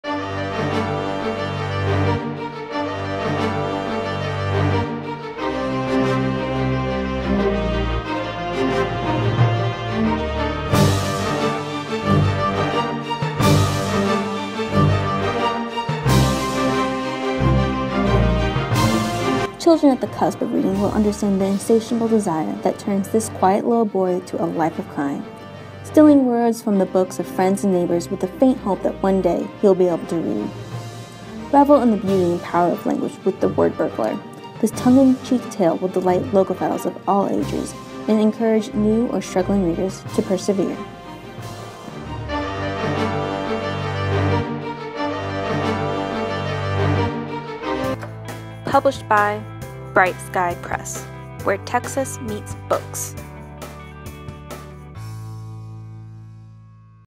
The Word Burglar
The Word Burglar by Chris Cander Illustrated by Katherine Tramonte

The Word Burglar reveals the beauty and power of language and helps children understand that without reading skills they become diminished. With a glossary of the Word Burglar’s purloined words and intriguing art that puts the power of books in proper perspective, this tongue-in-cheek tale will delight logophiles of all ages and encourage new or struggling readers to persevere.

Bright Sky Press
2365 Rice Blvd. Suite 202